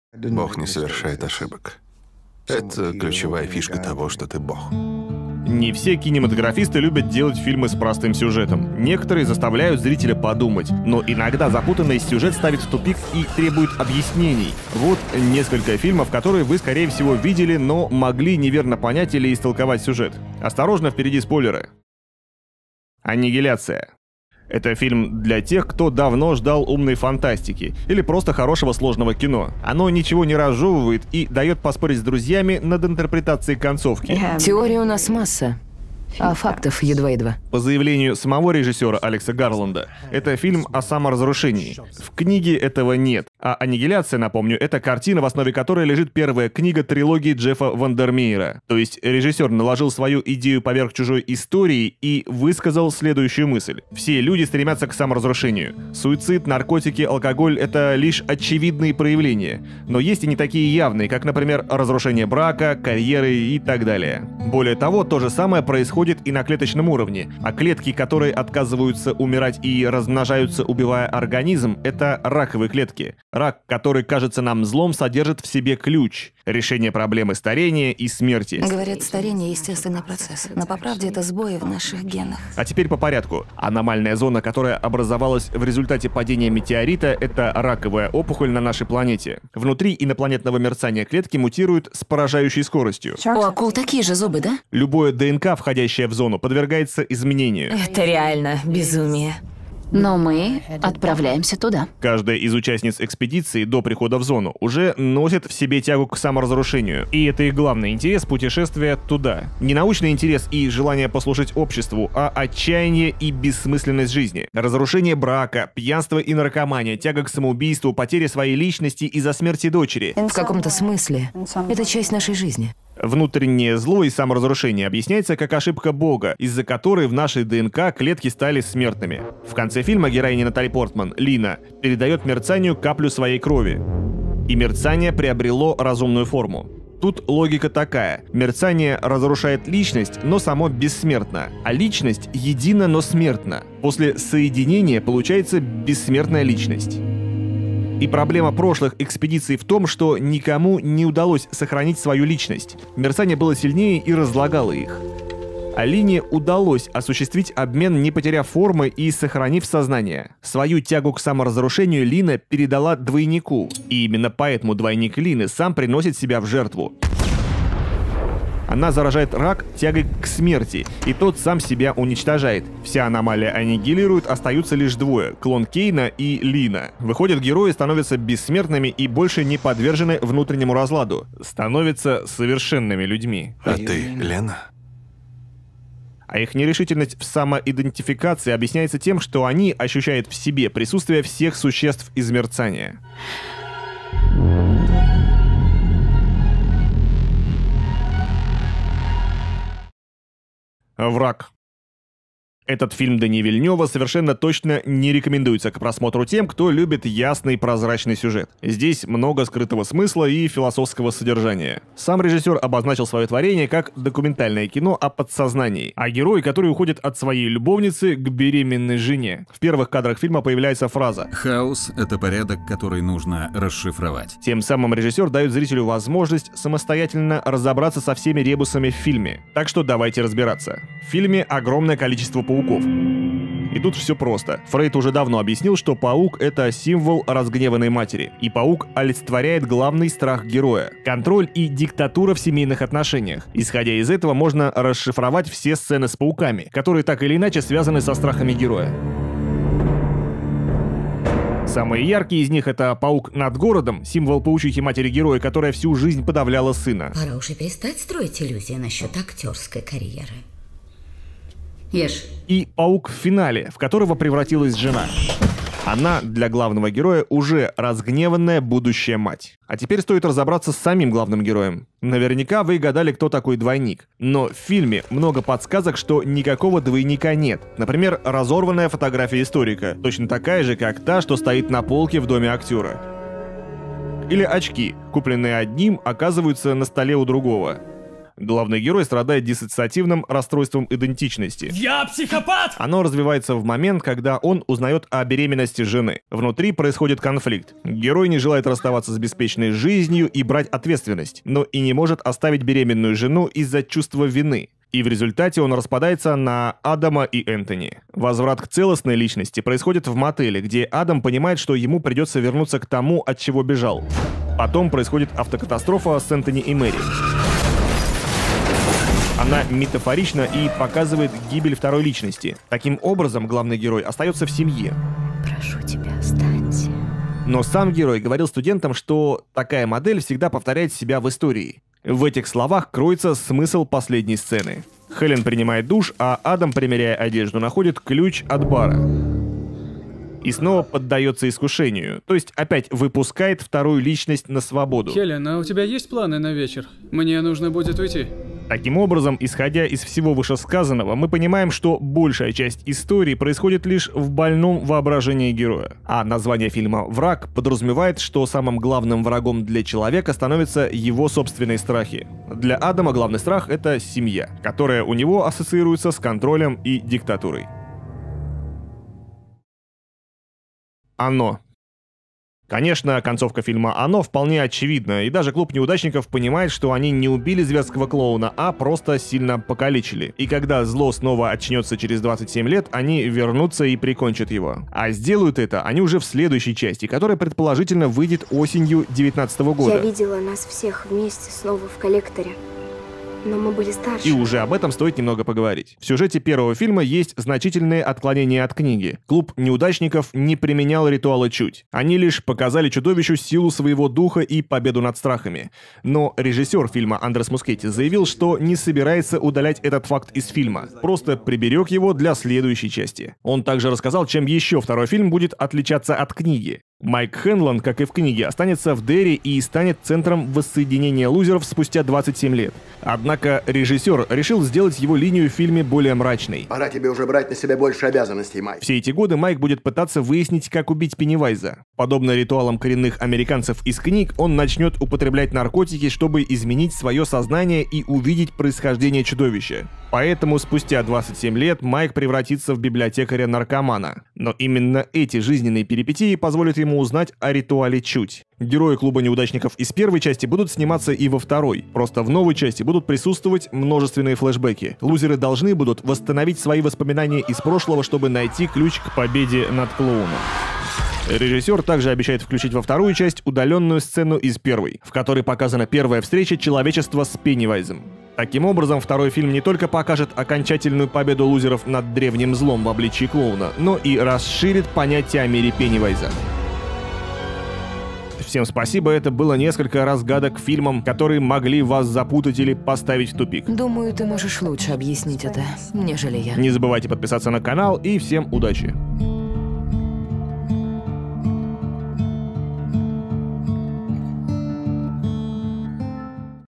Разгадки Фильмов Которые Понял не Каждый (аннигиляция, враг)
Вот несколько фильмов, которые вы скорее всего видели, но могли неверно понять или истолковать сюжет. ВПЕРЕДИ СПОЙЛЕРЫ!!!

1. Аннигиляция, 2018, IMDb: 7.10 | 0:23
2. Враг, 2013, IMDb: 6.90 | 3:20
3. Оно, 2017, IMDb: 7.50 | 6:10